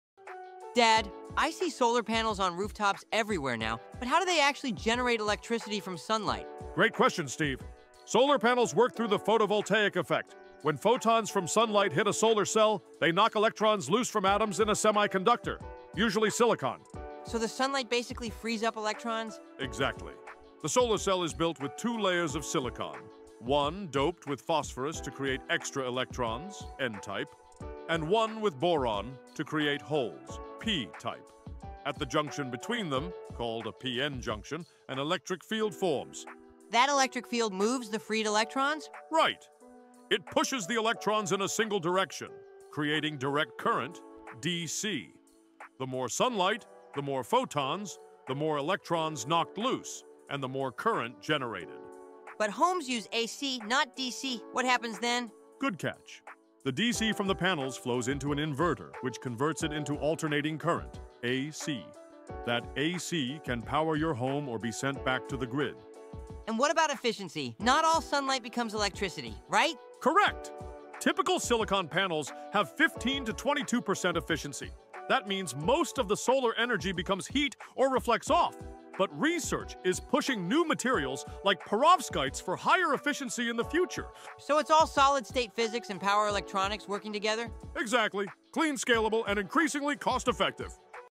How solar panels turn sunlight into electricity.
How do solar panels turn sunlight into electricity? Stan explains the photovoltaic effect, p-n junctions, and how your roof becomes a mini power plant. (Had to re-upload, there were some issues with the background video)